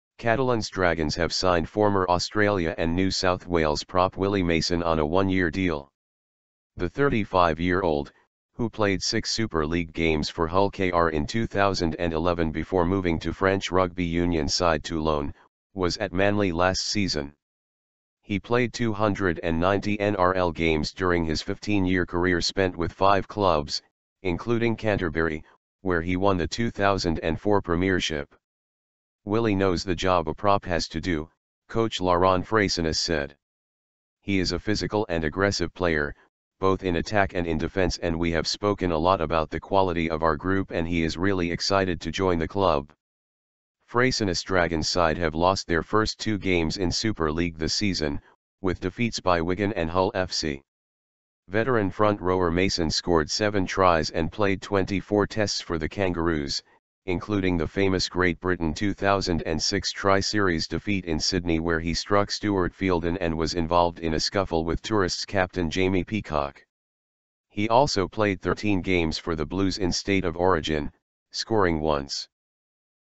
Willie Mason: Catalans Dragons sign veteran Australia prop
Catalans Dragons have signed former Australia and New South Wales prop Willie Mason on a one-year deal.

The 35-year-old, who played six Super League games for Hull KR in 2011 before moving to French rugby union side Toulon, was at Manly last season.

He played 290 NRL games during his 15-year career spent with five clubs, including Canterbury, where he won the 2004 Premiership.

"Willie knows the job a prop has to do," coach Laurent Frayssinous said.

"He is a physical and aggressive player, both in attack and in defence and we have spoken a lot about the quality of our group and he is really excited to join the club."

Frayssinous' Dragons side have lost their first two games in Super League this season, with defeats by Wigan and Hull FC.

Veteran front-rower Mason scored seven tries and played 24 tests for the Kangaroos, including the famous Great Britain 2006 Tri-Series defeat in Sydney where he struck Stuart Fielden and was involved in a scuffle with tourists captain Jamie Peacock.

He also played 13 games for the Blues in State of Origin, scoring once.